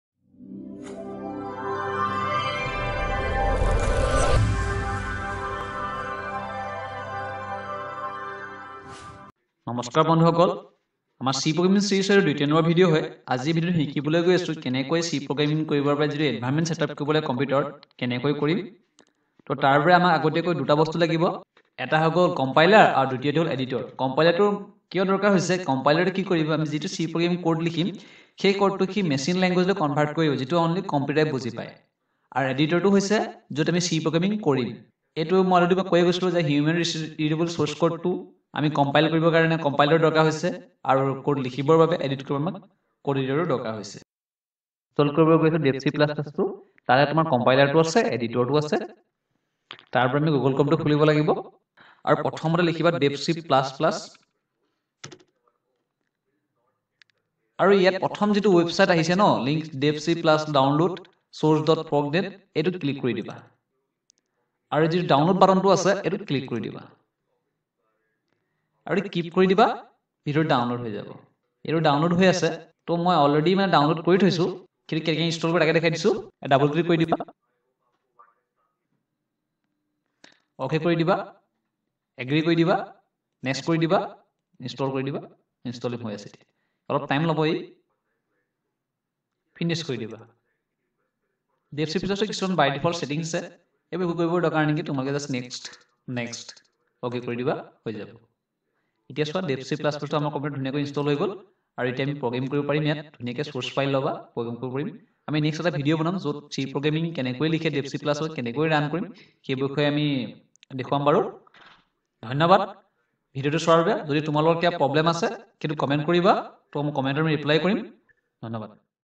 Environment setup of C ( Assamese ) || Tutorial 2
Step by step guide for Installation of Dev c++